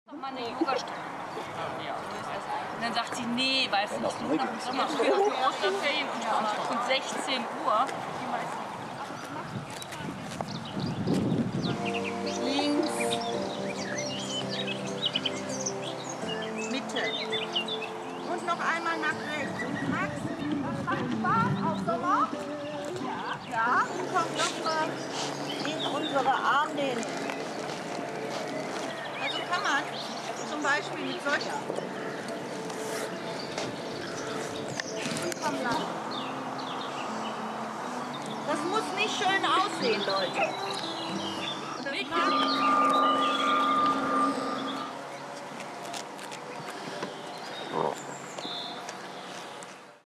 ویدئو؛ یوگا با لاما در آلمان
آلپاکا نوعی حیوان از تیره شترسان ها است که اغلب در آمریکای جنوبی زندگی می کند.…